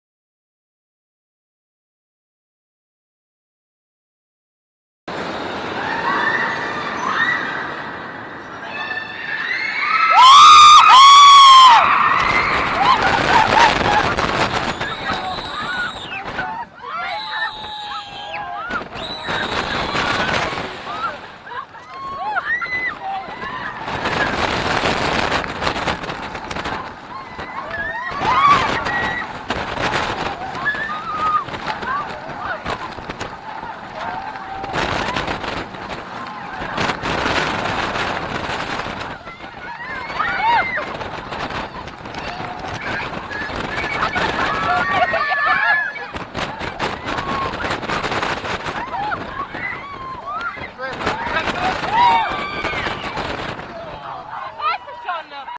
Der Schwur des KÄRNAN - Hansa-Park (Hyper Coaster) - Onride
Der 2015 im HANSA PARK eröffnete "Der Schwur des KÄRNAN"  ist der Weltweit einzigartige Hyper Coaster, welcher vor dem 67 m tiefen First Drop über einen 60 m hohen Rückwertsfreifall verfügt.

Viel Spaß mit dem Onride vom "Der Schwur des KÄRNAN".

Technische Daten von "Der Schwur des KÄRNAN"
Fallhöhe 67 m
Höhe 73 m
Anzahl der Inversionen 1
Länge 1235 m
Hersteller Gerstlauer Amusement Rides
Modell Hyper-Coaster
Sitzplätze Gesamt 48
Sitzplätze pro Einheit 16
Geschwindigkeit 127 km/h
Baujahr 2015